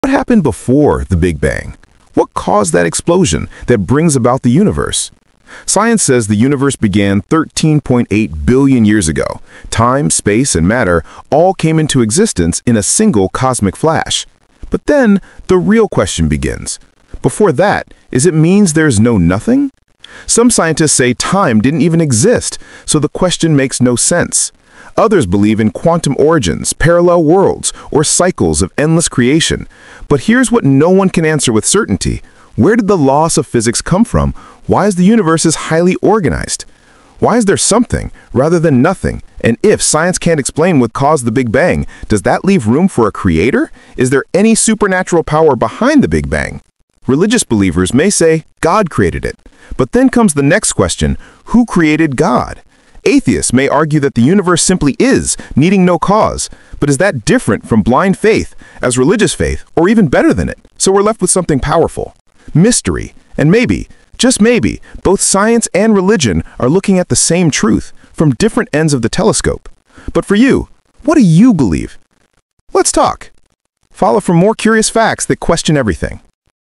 Before the Big Bang: God or Chance?
What existed *before* the Big Bang? Was it pure chance, quantum fluctuation… or the work of a Creator? This short dives into one of the greatest mysteries of existence — where science meets philosophy, and faith meets curiosity.
Explore the unknown. Question everything.